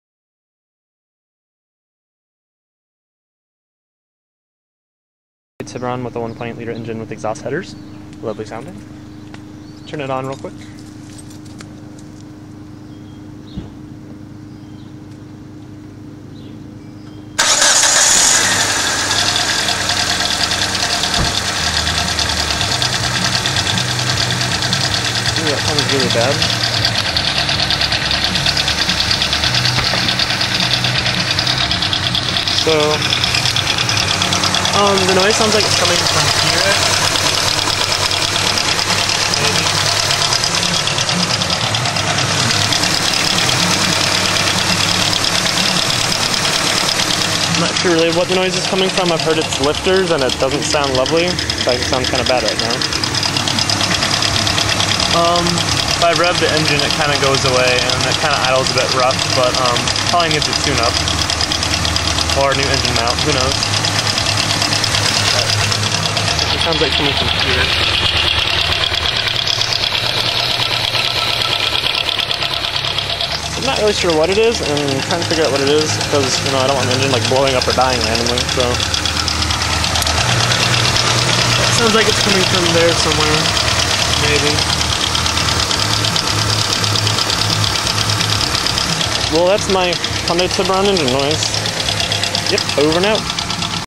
Hyundai Tiburon Engine Ticking / Knocking
So yeah, i bought the car with a bad exhaust manifold, replaced it with Exhaust Headers, and find this noise. =\ It's been with the car since i bought it about 5 months ago, i was told it was lifters, but i'm not so sure anymore. I hope it's not my engine dying.